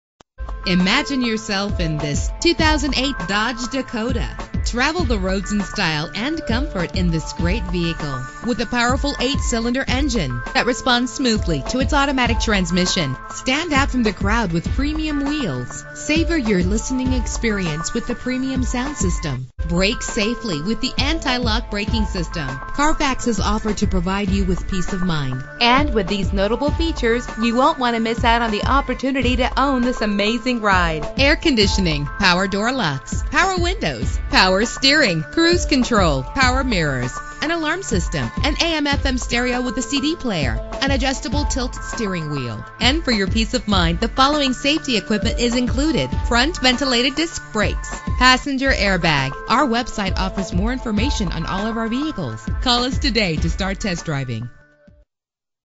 2008 Dodge Dakota - Wichita KS
Year: 2008
Make: Dodge
Model: Dakota
Trim: Laramie
Engine: 4.7 liter 8 cylinder 16 valve
Transmission: Automatic
Color: Black
Mileage: 33915
Address:
6000 Hattan Drive